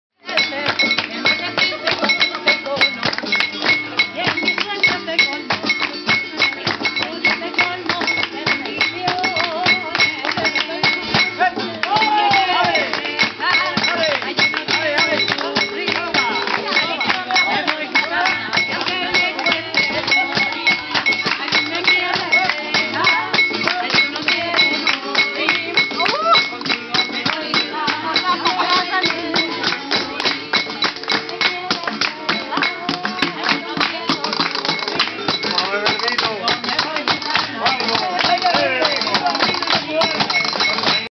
Coro Rociero La Peza - Fuente de La Gitana... 01
Comida en la fuente de La Gitana, en La Peza, del Coro Rociero del pueblo.